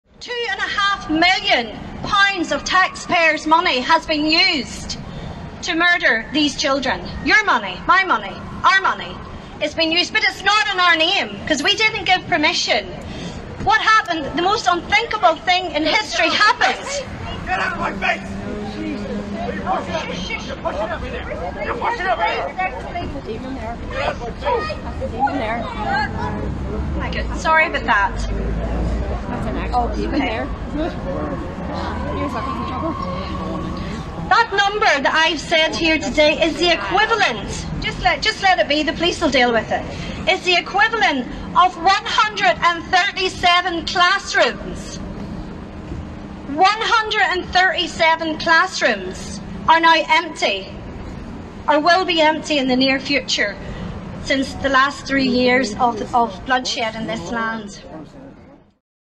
Man arrested for vicious bite attack at pro-life event in Belfast
A steward at a "LIFE CHAIN" was left with a serious bite to the neck after intervening to stop man assaulting a woman